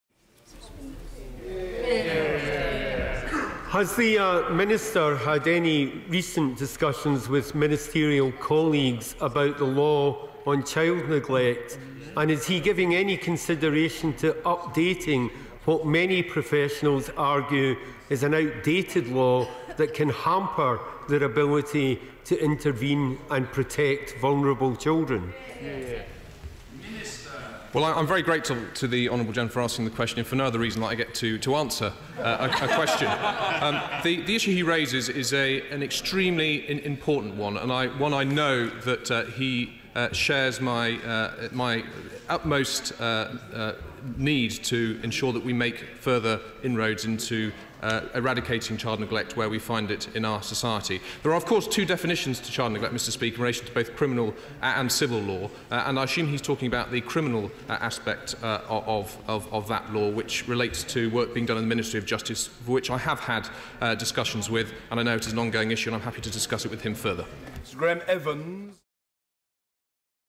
Steve McCabe MP questions Edward Timpson MP, Education Minister, on child neglect law
6 Janaury 2014